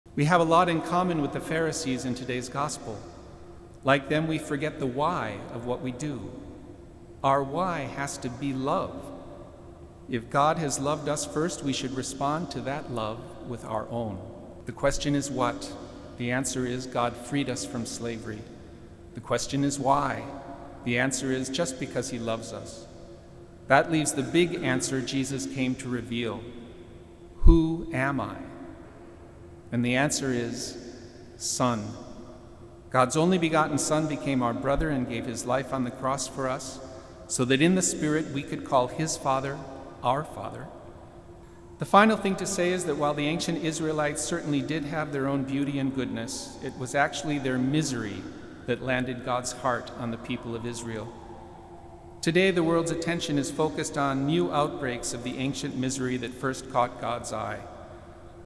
Bishop Mark Bartosic- "Who Am I? The Answer Is Son”
Taken from Bishop Bartosic's homily from October 29, 2023.